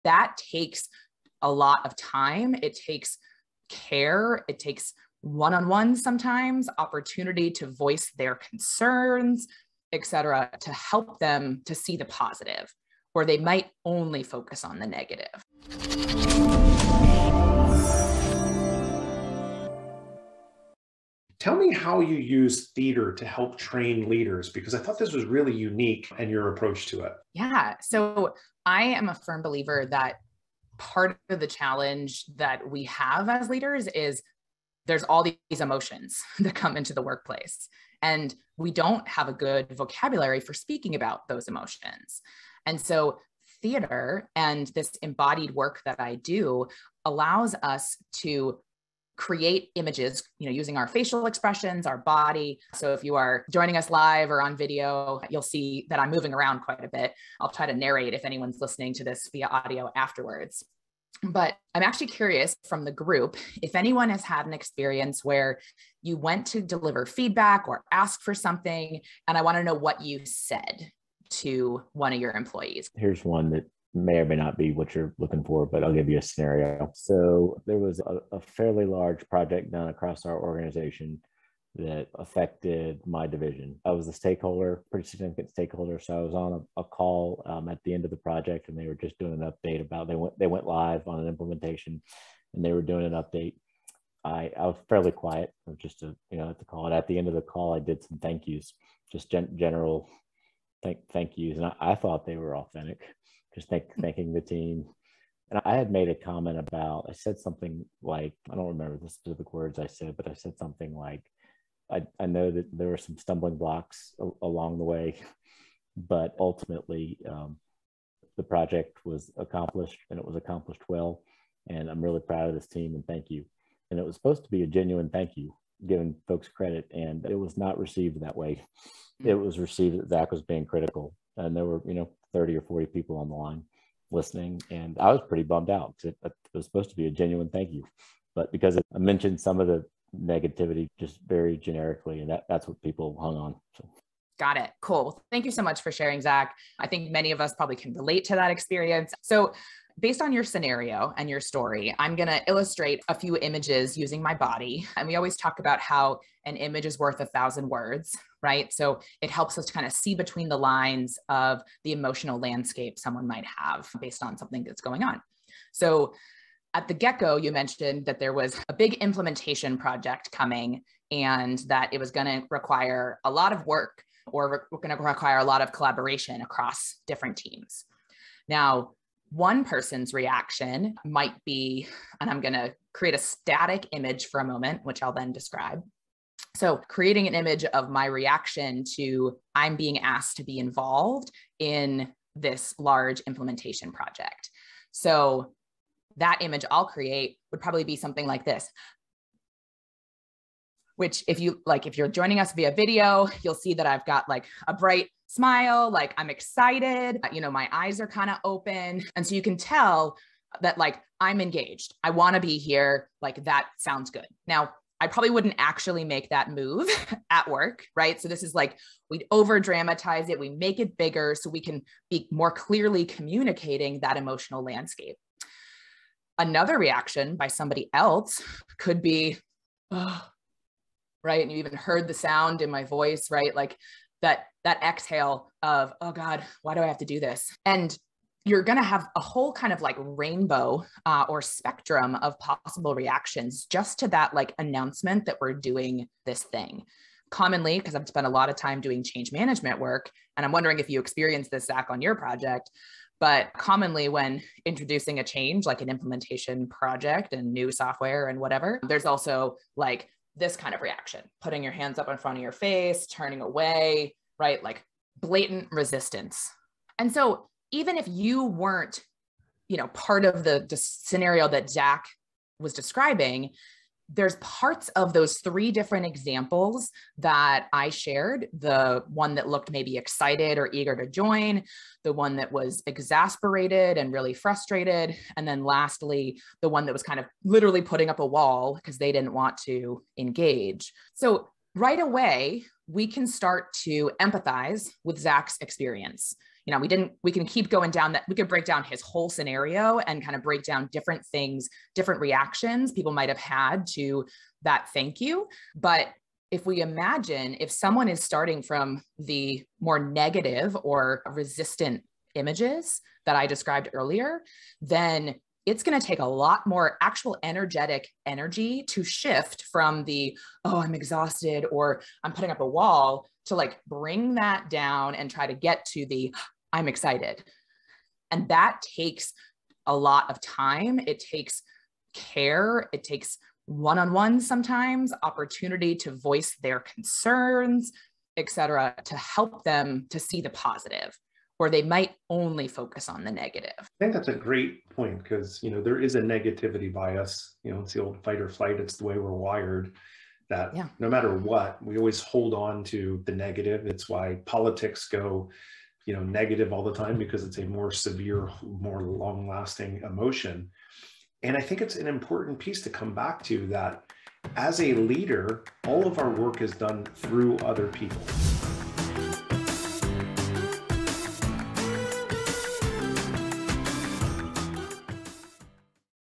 Emotional Intelligence 101 with Katie McLaughlin
As the saying goes, an image is worth a thousand words. This is especially true when it comes to communicating with others and providing feedback. The emotion displayed in the body language of both parties can often be strong indicators of their reaction to something or someone. It is important to pay attention to this emotional landscape as certain things may be missed if one were only focusing on verbal communication alone.

The power of body language should not be underestimated, as it has been proven that people are naturally drawn more toward positivity than negativity. In fact, research indicates that humans have a more intense and lasting reaction when exposed to negative stimuli rather than positive ones, which can sometimes cloud our judgment and lead us astray from our desired outcome.